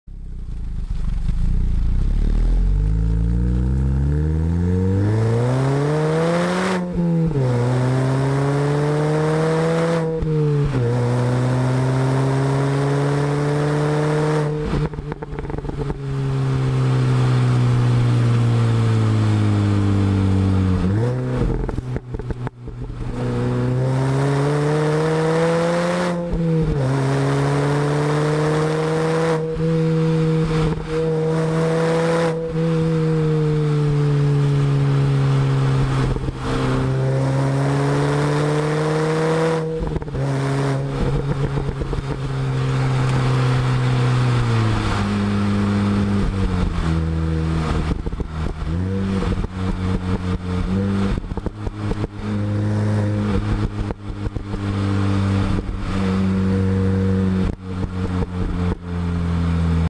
2004 MINI Cooper S JCW Convertible - On Board, with GoPro and external mic.
Here's a quick on board clip of my R52 JCW, I've used a Go Pro Black Edition and external mic places above the exhaust. My next video will feature a dual mic setup so you can hear the supercharger too!